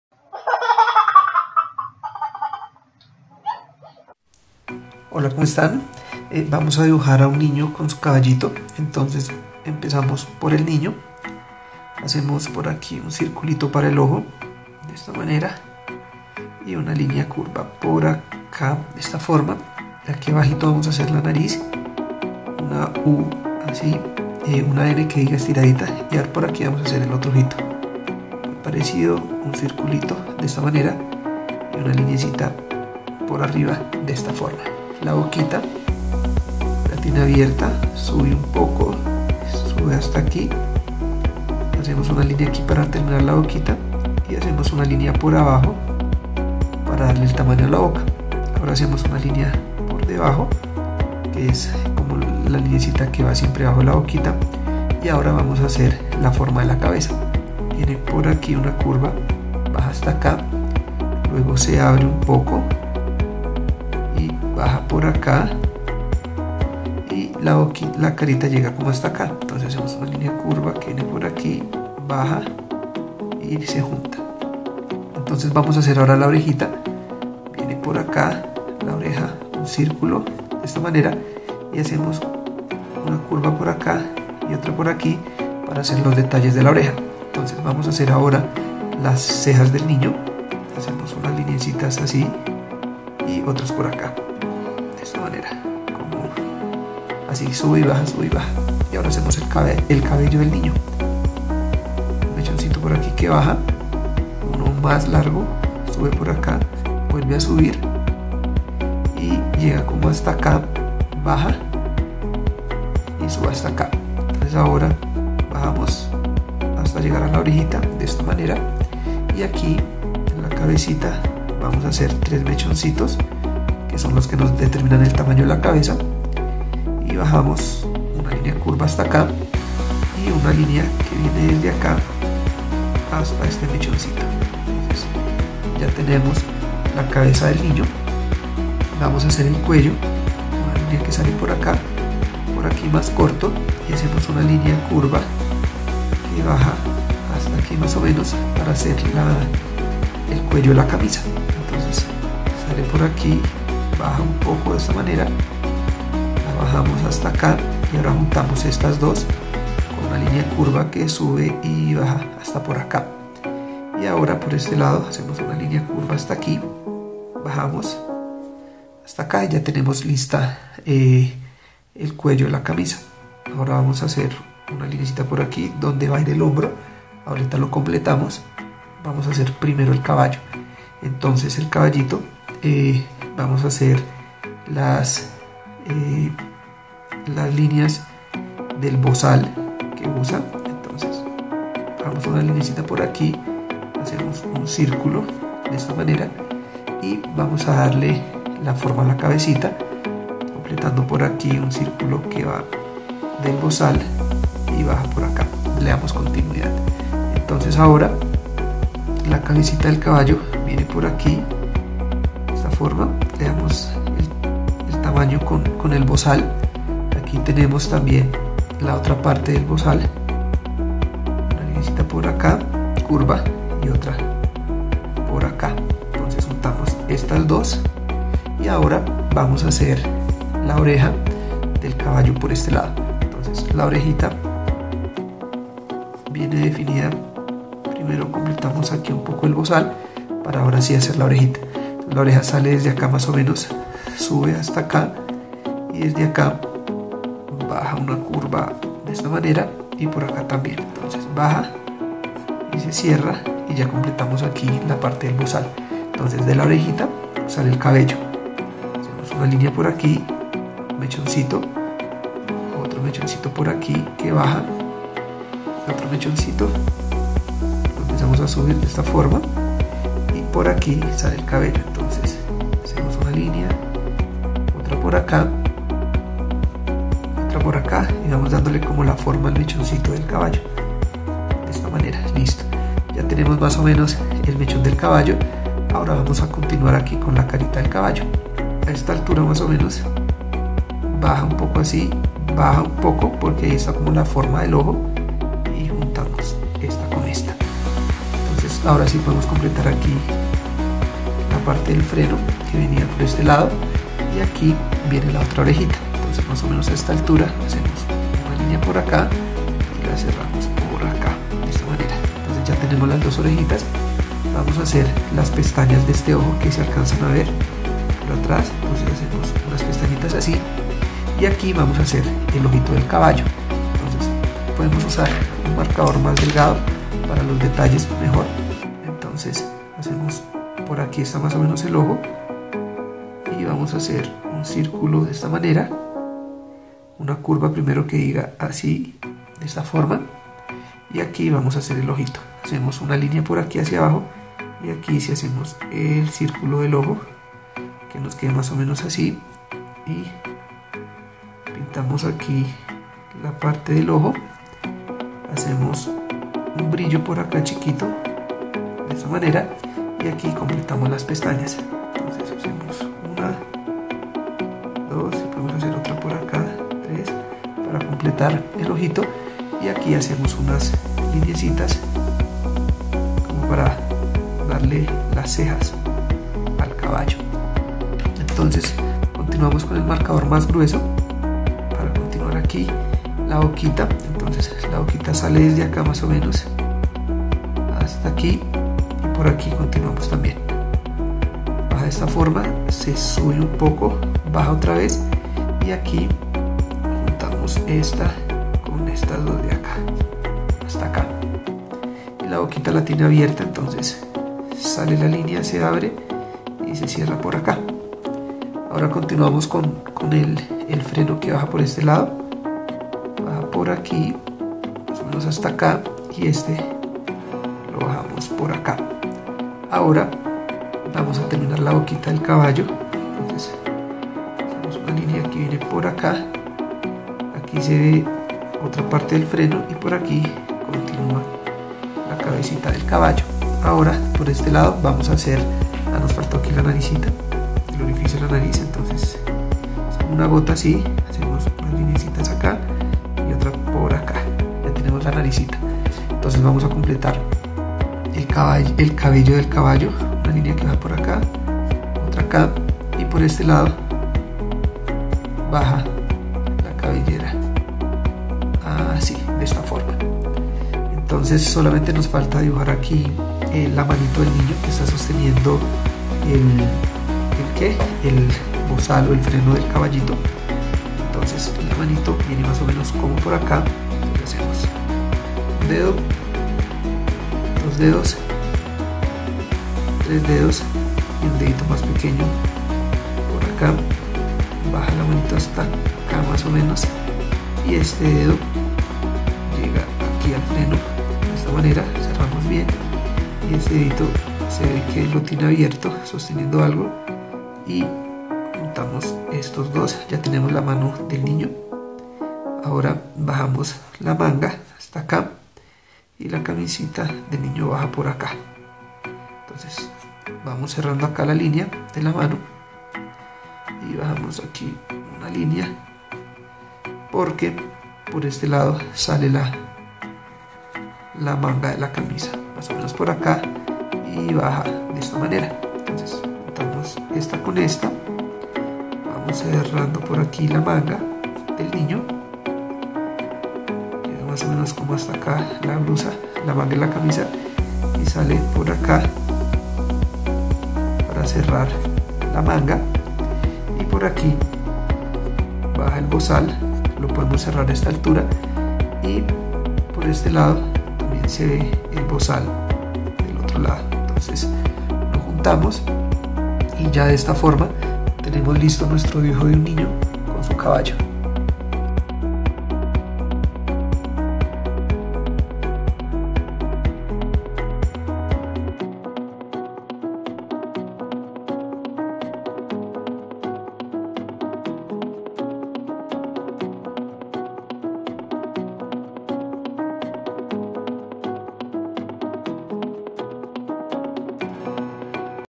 Como dibujar a UN NIÑO CON SU CABALLO | Dibujos faciles
Aprende cómo dibujar un caballo paso a paso y de la forma más fácil. Tutoriales de dibujo de caballos para que aprendas a dibujar dibujos fáciles.